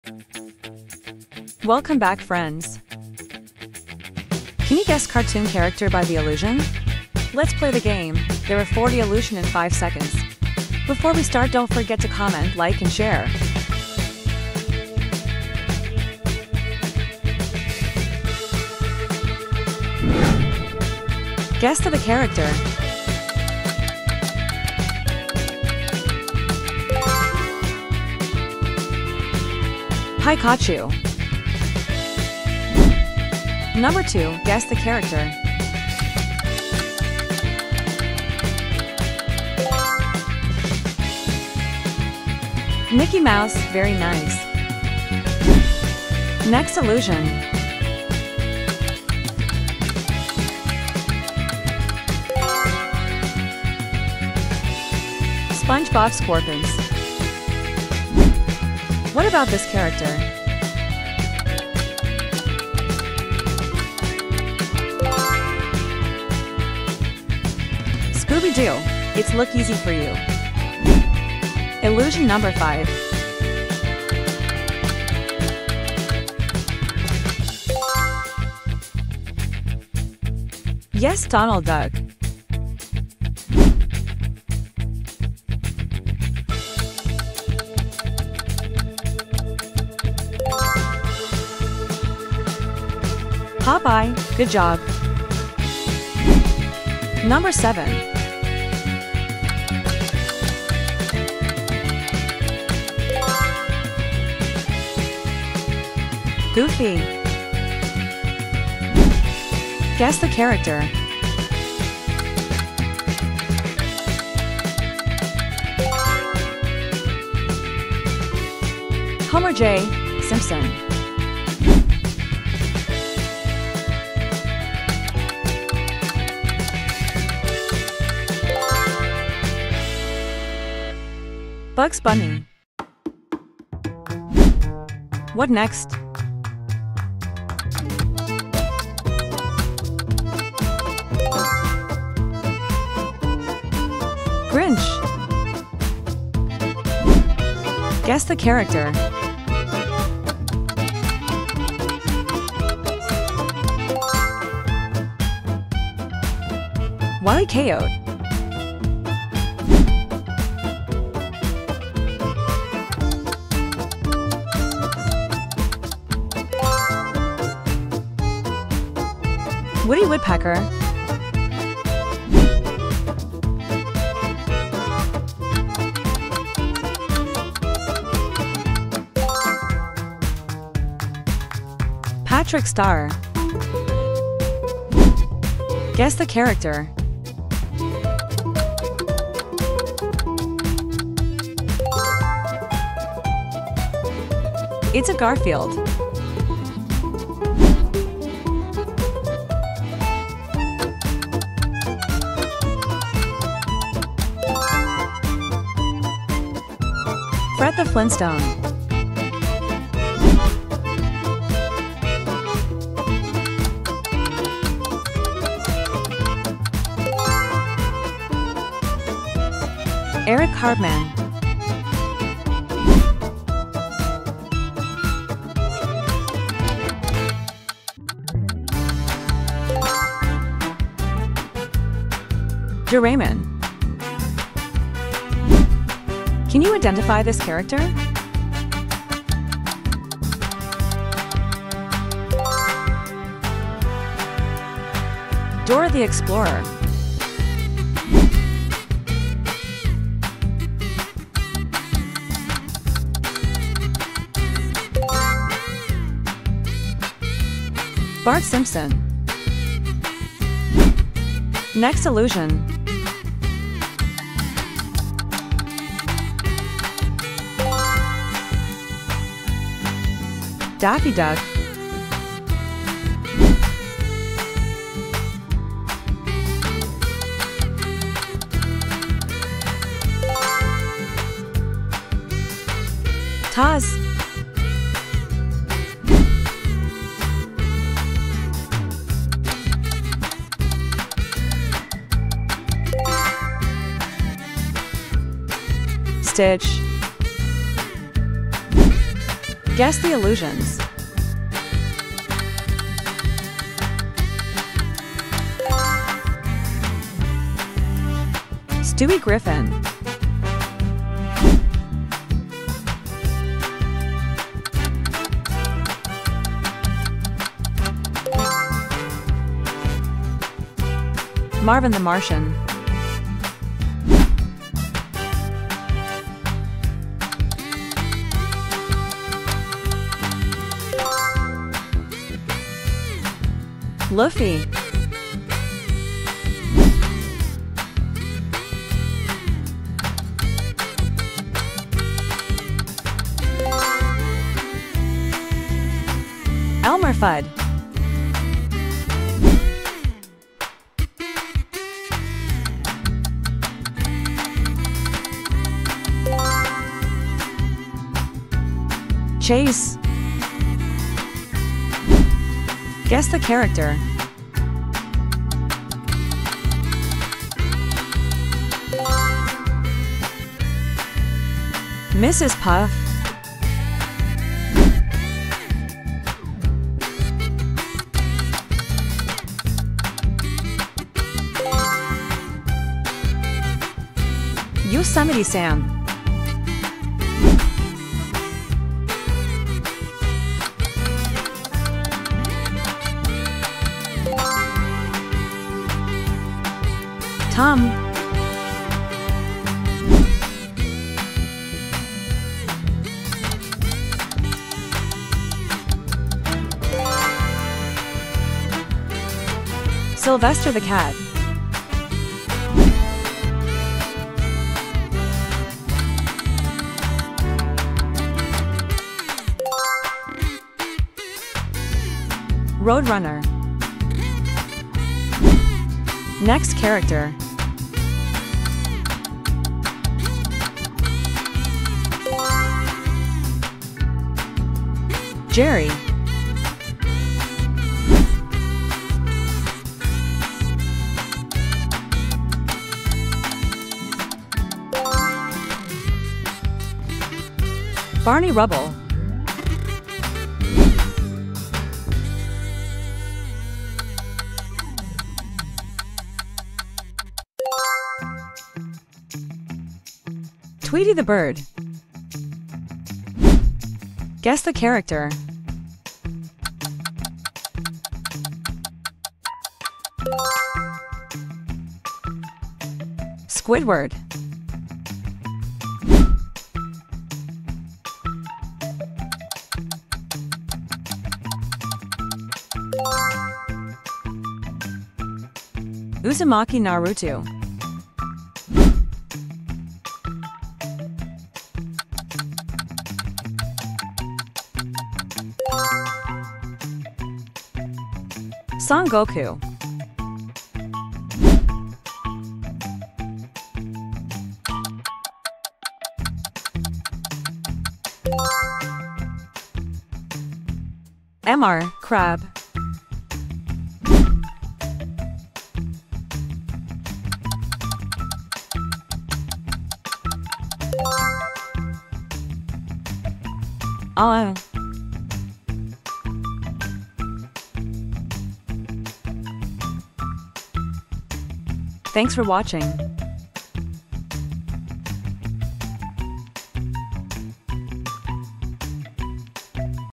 Guess the Character by the Illusion|Special Cartoon Edition
Welcome back to lakiluk quiz. Guess the Cartoon character by the illusion. How good your eyes? Let's play the quiz